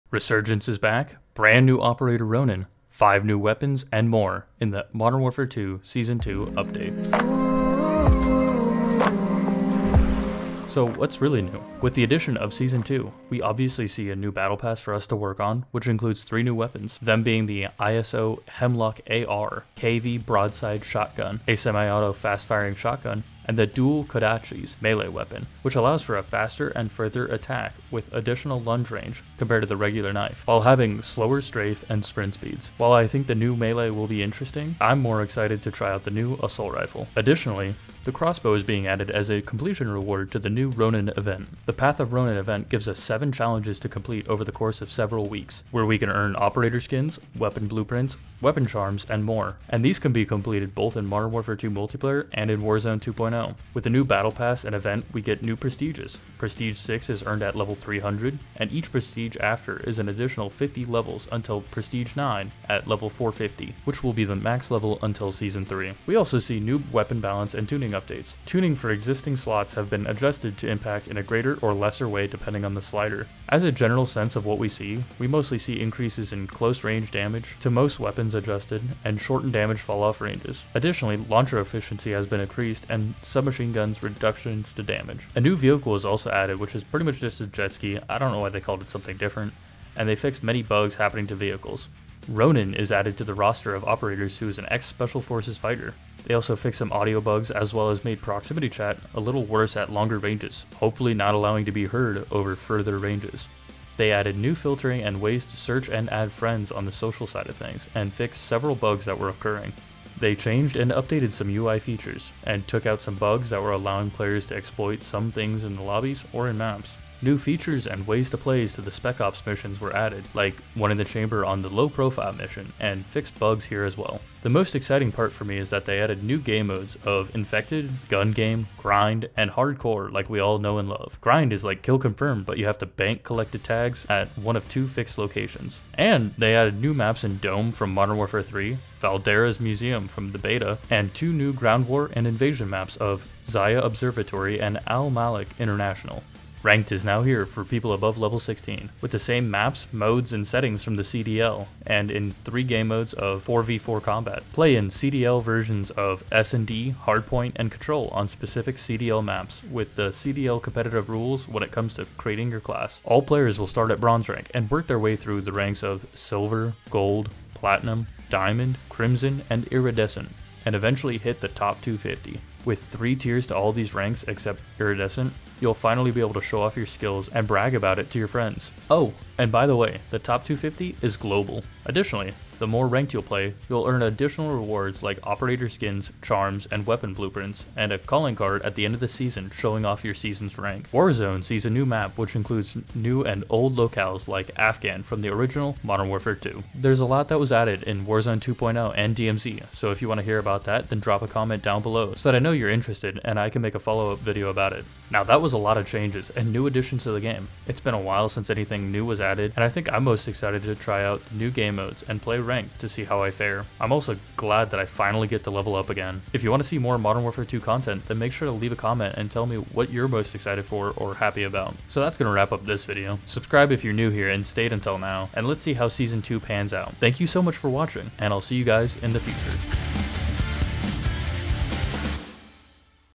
Call of Duty: MW2's Brand New Season - Here's What You Need to Know!
Join me as we dive into all the new information that you need to know about the NEW Season 2 update to Call of Duty: Modern Warfare 2.

The NEW update brings cool new weapons, operator, skins, and more! With the addition of new modes and maps along with the NEW Ranked mode there is sure to be something that you will enjoy. Maybe join me as I hop back into MW2 and explore whats new.